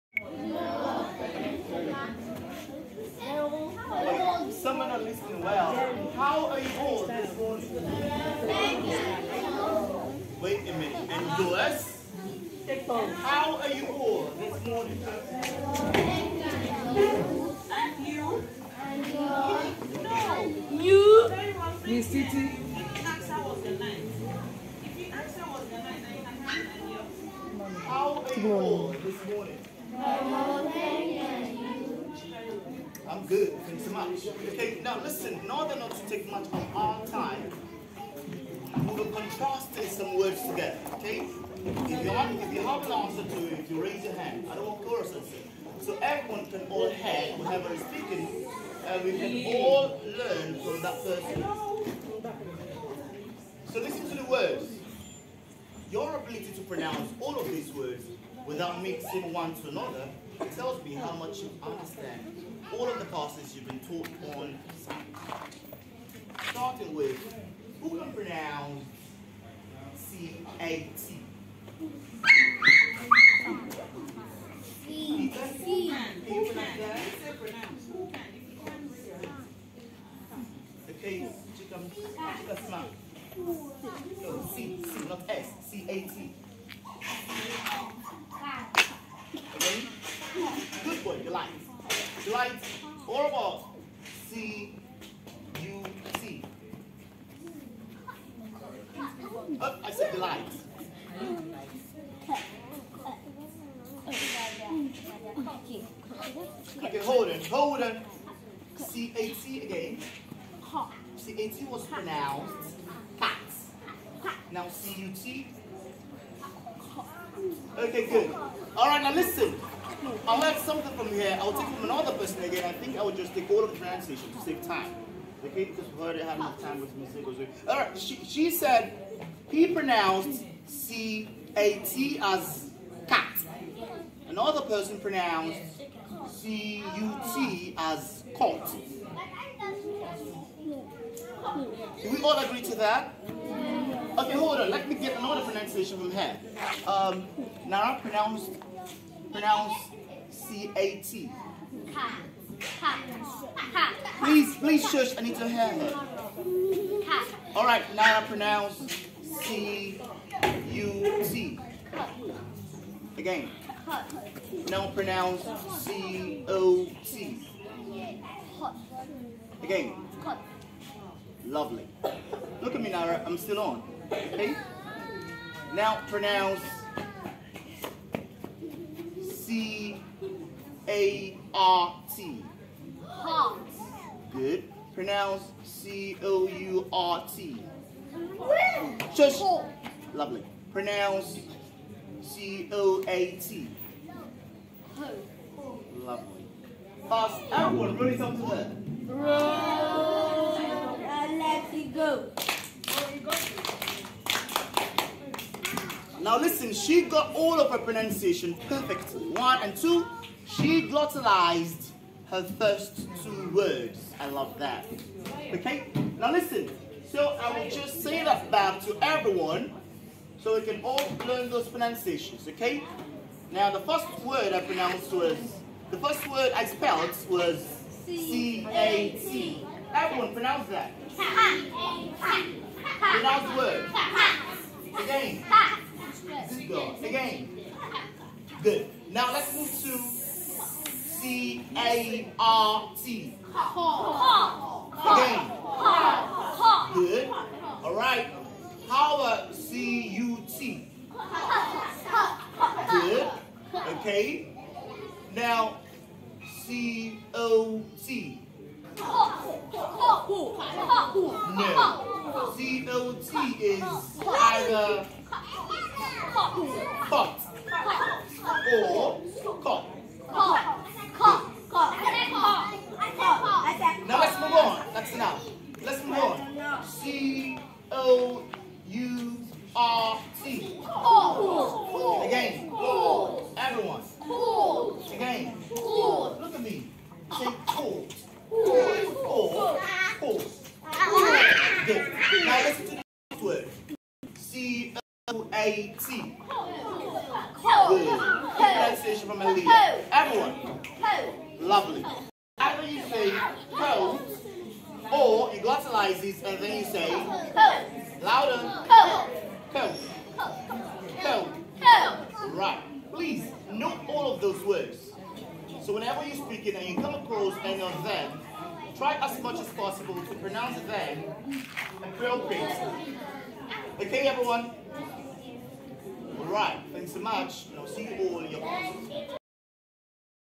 Learn the appropriate articulation of these words cat cut cot cart court coat.
Understanding, measuring and approving the qualities and effectiveness of learners' speech is essential after instillation of sound English knowledge in them.

Learn the appropriate articulation of these words cat cut cot cart court coat as this pupil from @shorefieldms contrast those above words distinctively.

Do you want speech transformation?